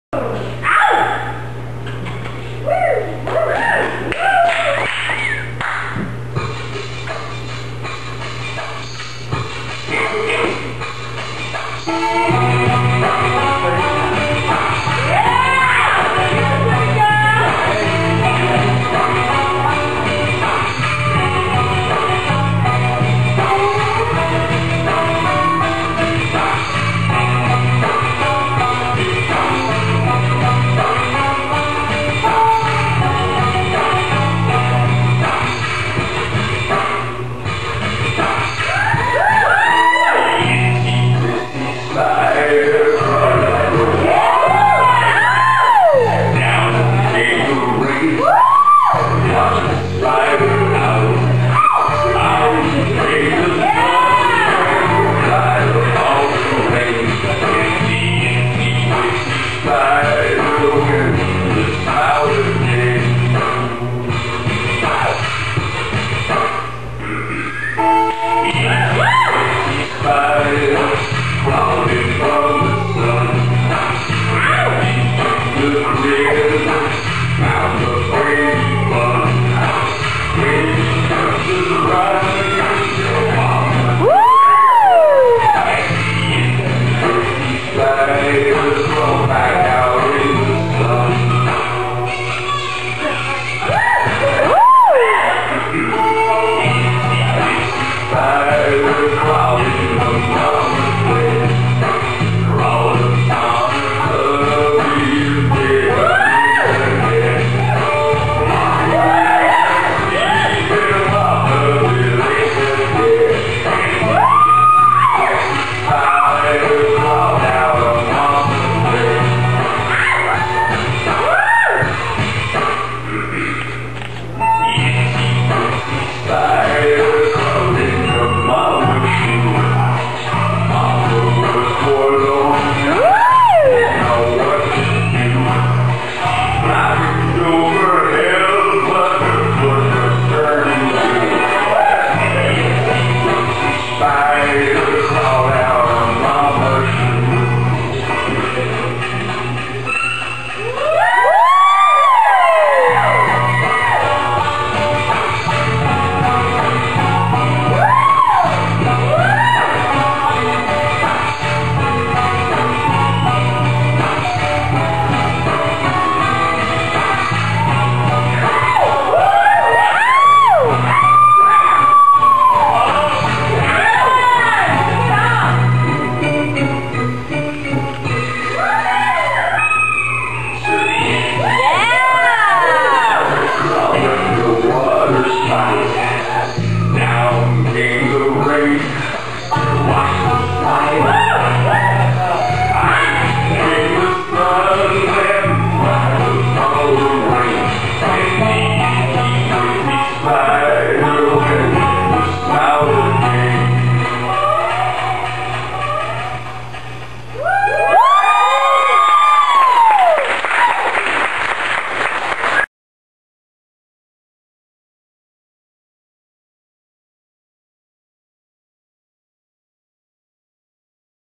Jezzebel spider dance, The Desert Dolls Burlesque
Jezzebel brings a gothic twist to a saucy belly dance for our things that go bump and grind in the night show Oct. 15th 2011